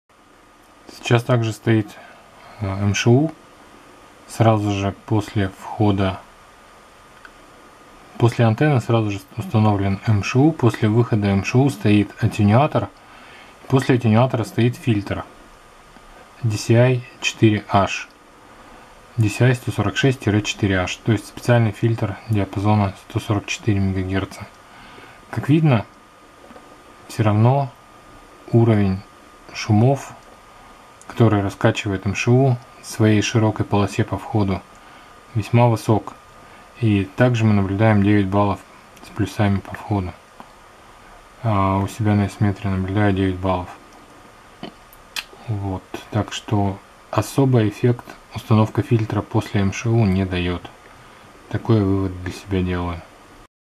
Тест 2: Антенна - МШУ - АТТ - фильтр
Проверяю на примере приема Эхолинка за 70км связку:
Антенна - МШУ - АТТ - фильтр